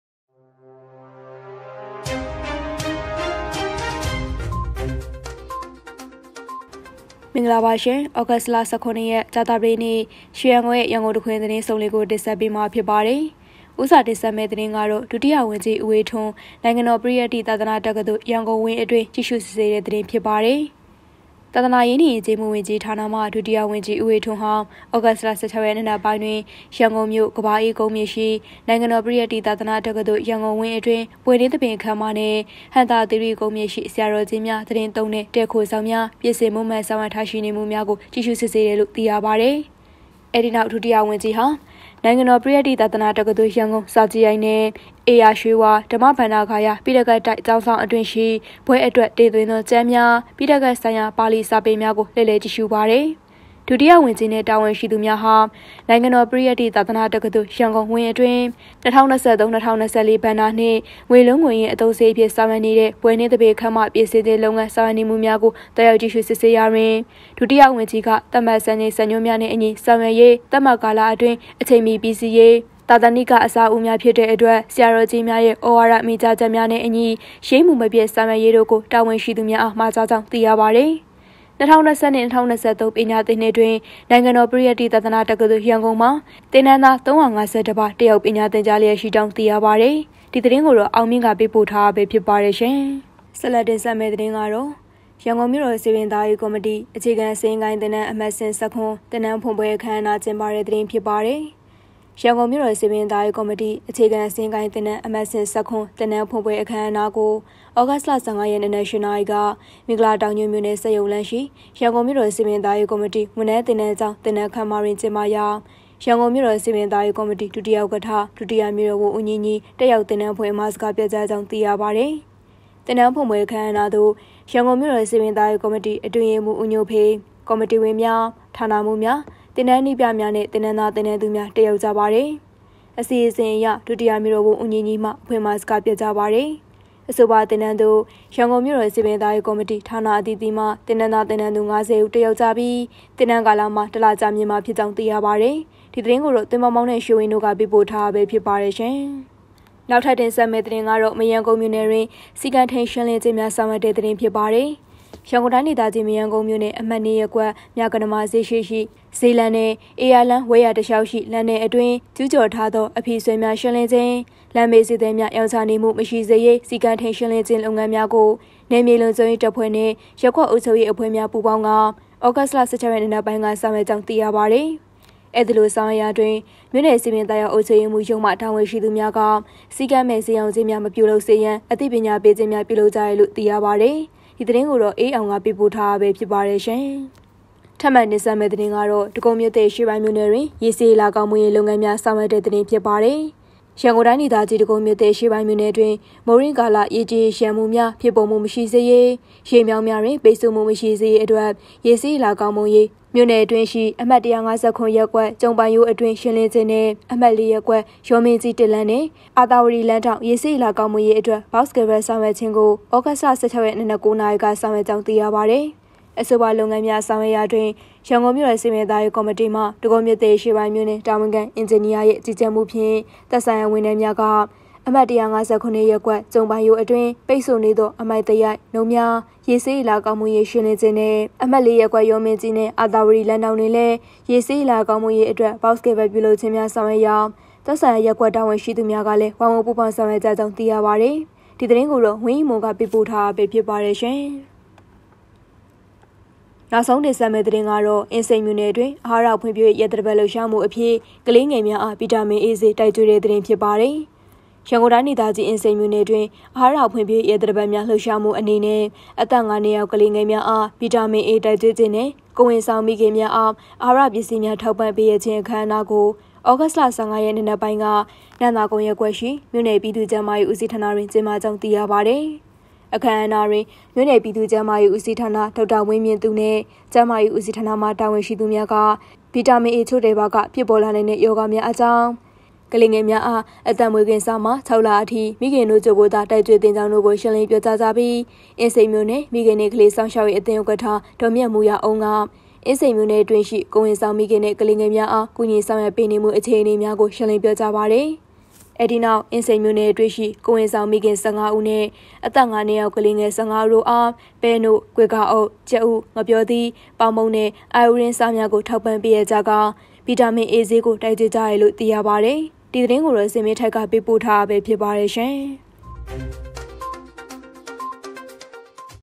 ရန်ကုန်တစ်ခွင် သတင်းစုံလင်( ၁၇.၈.၂၀၂၃ )
ရန်ကုန်တစ်ခွင် သတင်းစုံလင်
( ၁၇.၈.၂၀၂၃ )
- ဒုတိယဝန်ကြီးဦးအေးထွန်း နိုင်ငံတော်ပရိယတ္တိ သာသနာ့တက္ကသိုလ်(ရန်ကုန်)ဝင်းအတွင်း ကြည့်ရှုစစ်ဆေး
- ရန်ကုန်မြို့တော်စည်ပင်သာယာရေးကော်မတီ၊ အ‌ခြေခံစာရင်းကိုင်သင်တန်း အမှတ်စဉ် (၁၇) သင်တန်းဖွင့်ပွဲအခမ်းအနား ကျင်းပ
- မရမ်းကုန်းမြို့နယ်၌ စည်းကမ်းထိန်းရှင်းလင်းခြင်းများ ဆောင်ရွက်
- ဒဂုံမြို့သစ်(အရှေ့ပိုင်း)မြို့နယ်တွင် ရေစီးရေလာကောင်းမွန်ရေးလုပ်ငန်းများ ဆောင်ရွက်
- အင်းစိန်မြို့နယ်၌ အာဟာရဖွံ့ဖြိုးရေးရက်သတ္တပတ်လှုပ်ရှားမှုအဖြစ် ကလေးငယ်များအား ဗီတာမင်အေဆေးတိုက်ကျွေး